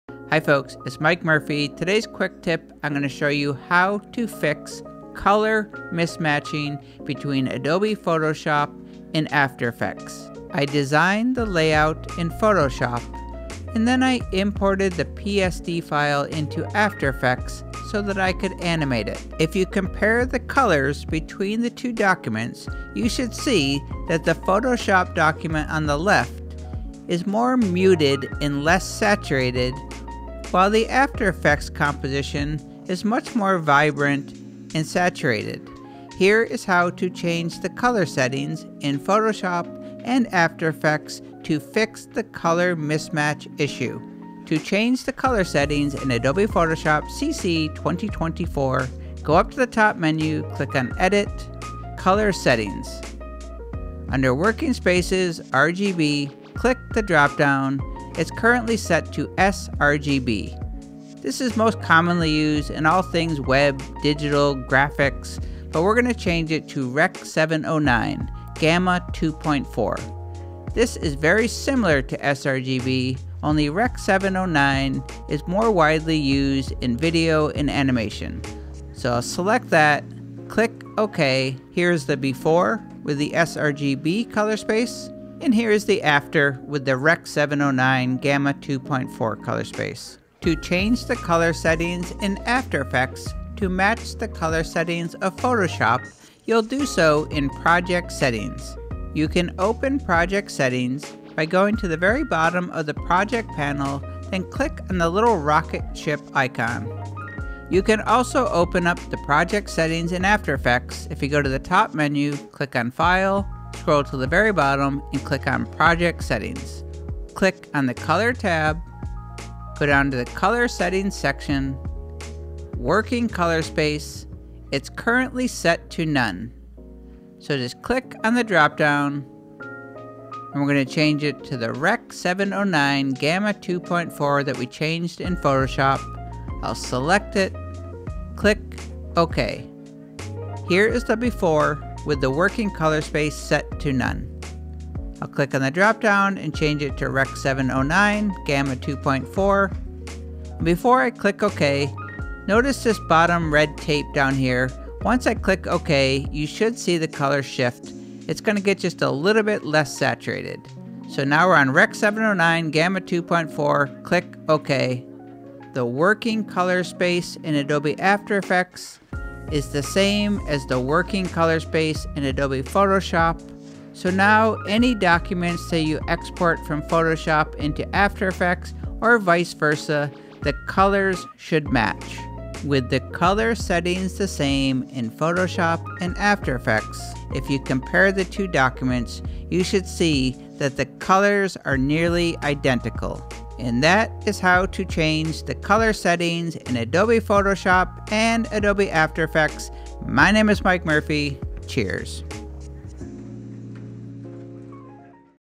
Fix Color Mismatch in Photoshop + After Effects
Photoshop + After Effects: How To Fix Color Mismatches

This tutorial will show you how to fix color mismatches between Adobe Photoshop CC 2024 & Adobe After Effects CC 2023.

Working Color Space Used in Tutorial:Rec.709 Gamma 2.4

Keyboard Shortcuts
Shift + Cmd/Ctrl + K (Color Settings PS)Option/Alt + Shift + Cmd/Ctrl + K (Project Settings AE)

Chapters:
00:00  Intro: About The Tutorial
00:19  Comparing Color in Photoshop vs. After Effects
00:44  Color Settings in Photoshop
01:01  Select Rec.709 Gamma 1.4
01:21  Before & After Comparison
01:29  Change Color Settings in After Effects
01:56  Project Settings (Color Tab)
02:20  Before & After in After Effects
03:02  Comparison Between PS & AE Files

How To Change Color Settings in Adobe Photoshop CC 2024:
1. Open Photoshop
2. Go to the ‘Edit’ Menu
3. Go to ‘Color Settings’
4. Go to ‘Working Spaces’ section
5. Click the drop-down next to ‘RGB’
6. Change from sRGB to Rec.709 Gamma 2.4
7. Click ‘OK”

How To Change Color Settings in Adobe After Effects CC 2023:
1. Open Adobe After Effects
2. Go to the bottom of the Project Panel
3. Click on the ‘Spaceship’ icon or,
4. Go to the File Menu
5. Select ‘Project Settings’
6. Click on the ‘Color’ tab
7. Go to ‘Color Settings’ section
8. Go to ‘Working Color Space’
9. Click the drop-down menu
10. Change from ’None’ to ‘Rec.709 Gamma 2.4’
11. Click ‘OK